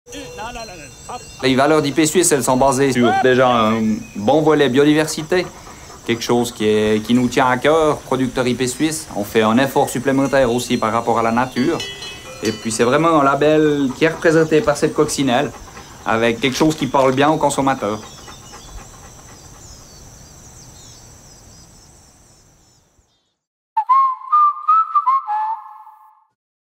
Produzione di carne IP-SUISSE
Come vivono gli animali tenuti secondo gli standard IP-SUISSE? Scopri di più sul marchio e sui prodotti al sito migros.ch/ipsuisse.

Per contadini come Joël Scheidegger del Canton Vaud la pluriennale collaborazione tra Migros e IP-SUISSE è importante. Cosa significa il legame con il marchio e con il grande gruppo industriale per lui e per i suoi bovini lo si può scoprire nella breve presentazione sulla produzione di carne secondo gli standard IP-SUISSE.